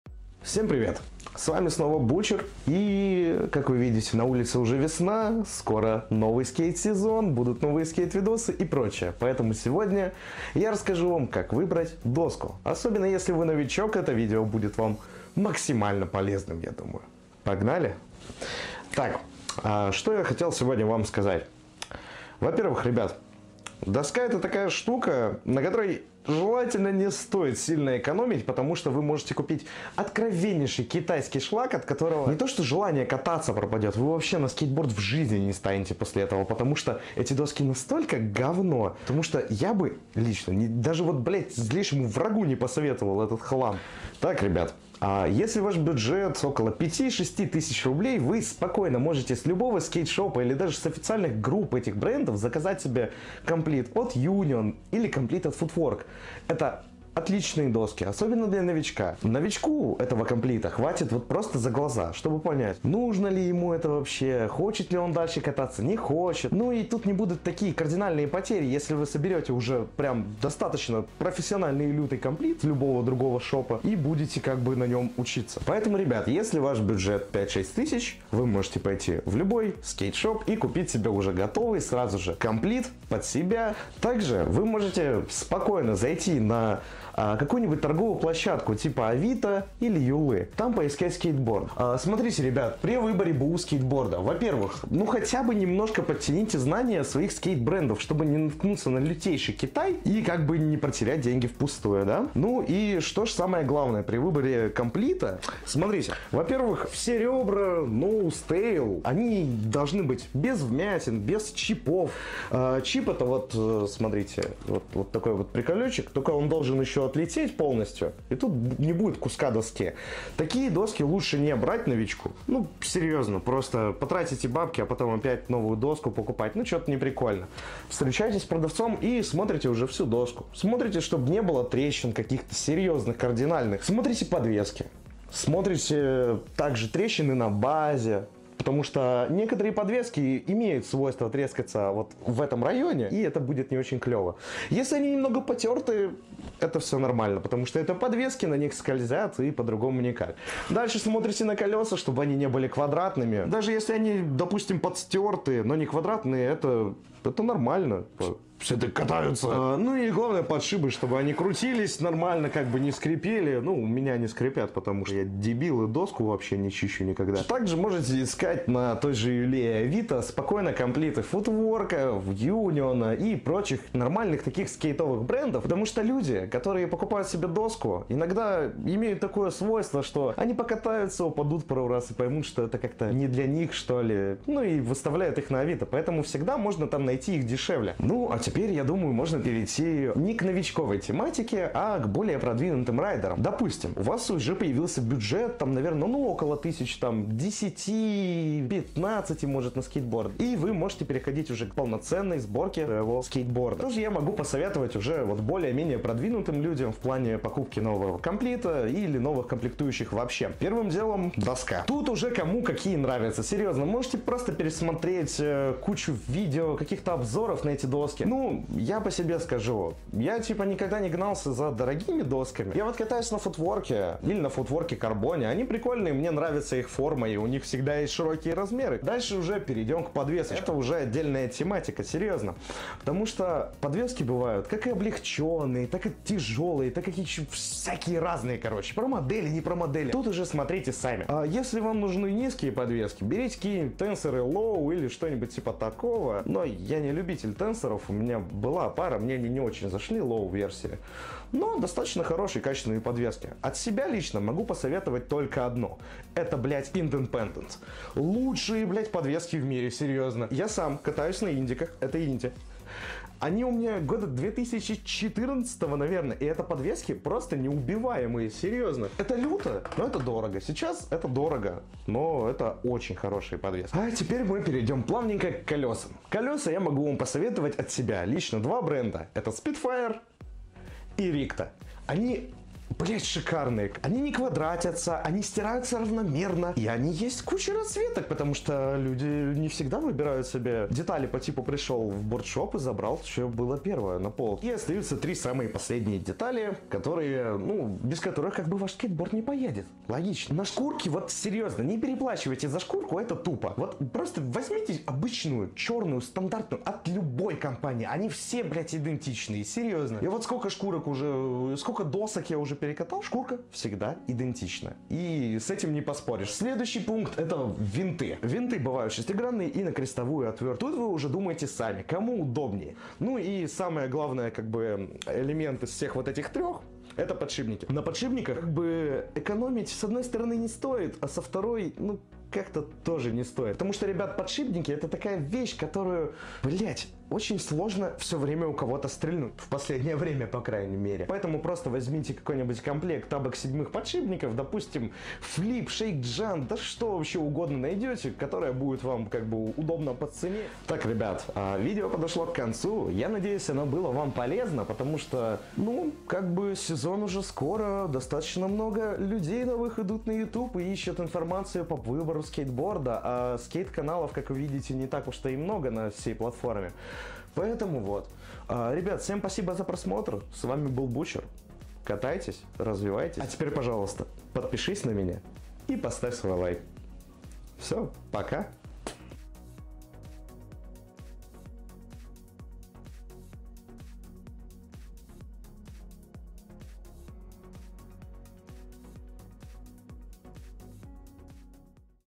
КАК ВЫБРАТЬ ПЕРВЫЙ СКЕЙТБОРД?КАКОЙ СКЕЙТ КУПИТЬ НОВИЧКУ?
Всем привет,в этом видео я расскажу как правильно выбрать свой первый скейтборд,а так же как выбрать Б/У скейтборд,надеюсь это видео вам помогло,спасибо большое за просмотр)